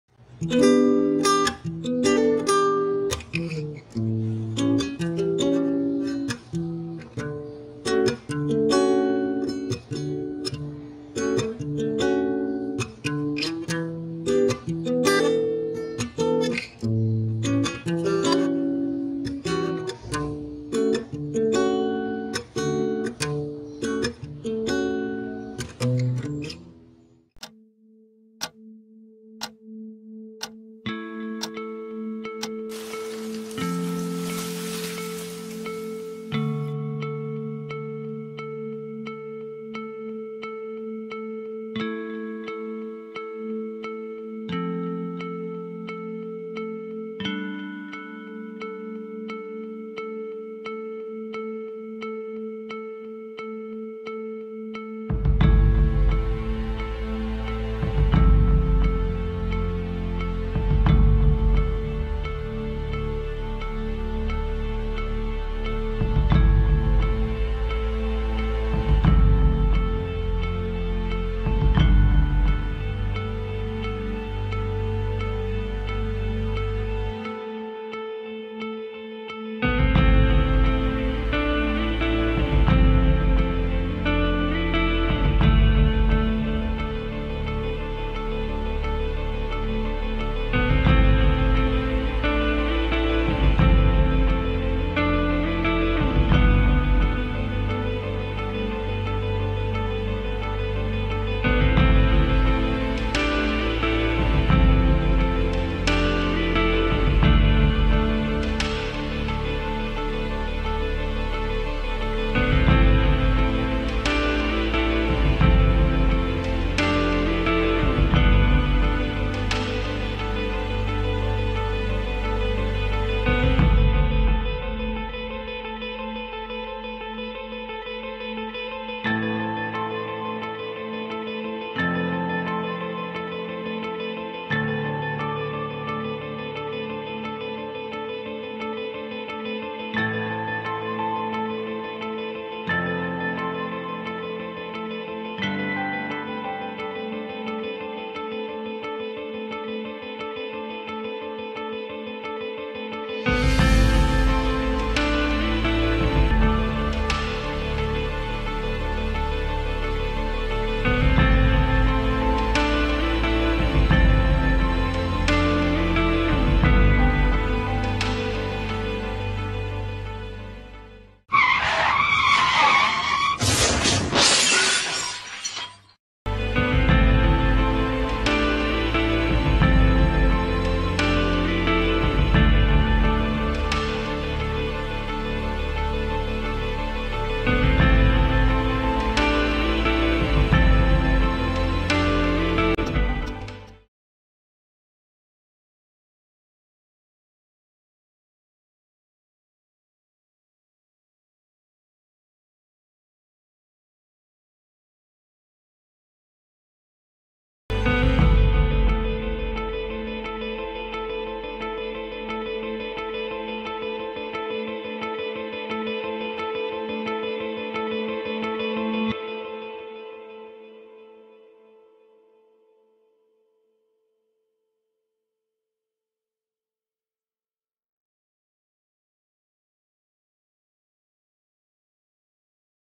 Time (Oras) | Experimental Short Film
Eto nga po pala yung short video para sa subject namin na Media Information Literacy. Sana magustuhan niyo yung simpleng short film namin. MARAMING SALAMAAAAT!!!